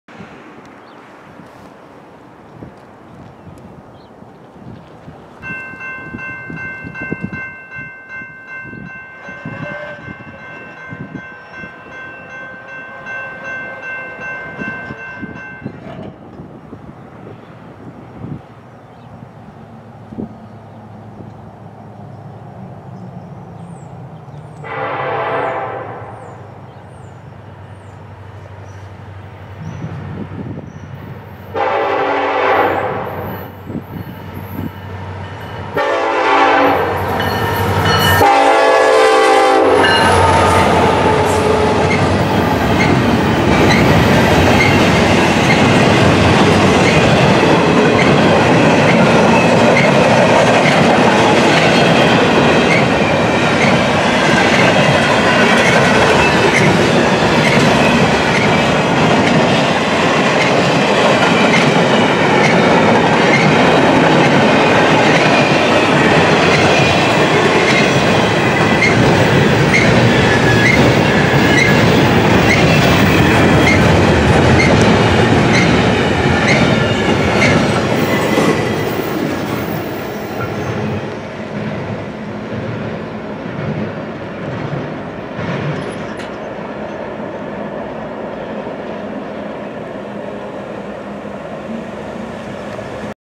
NORFOLK SOUTHERN EMD SD70ACE Single Southbound Intermodal Container Train
A southbound Intermodal Container Lead by Norfolk Southern EMD SD70ACE #1024 Engine passing at Milepost Rosemont.